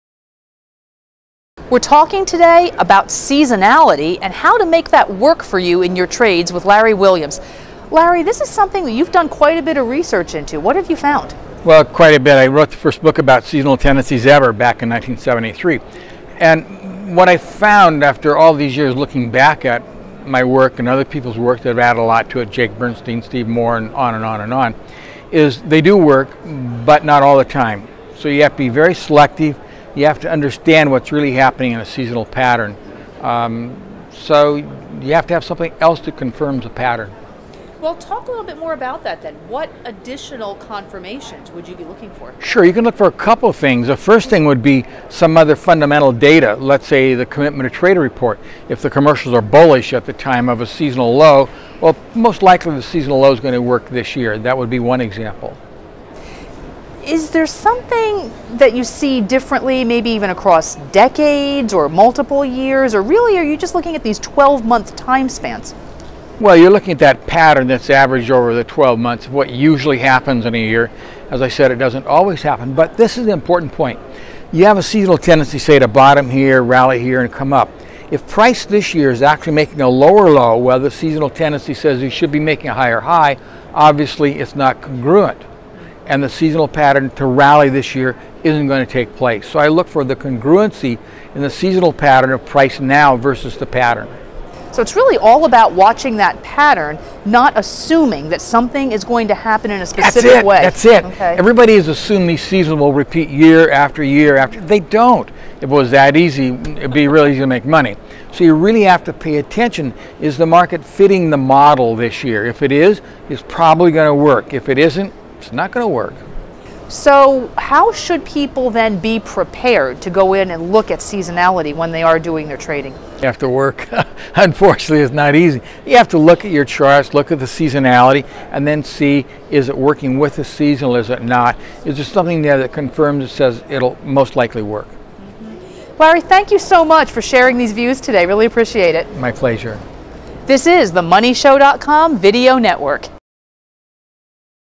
Easy Way to Get Burned by Seasonality
No seasonal patterns are set in stone, warns Larry Williams, but traders can look for important validation signals before executing seasonal trades and vastly improve their odds.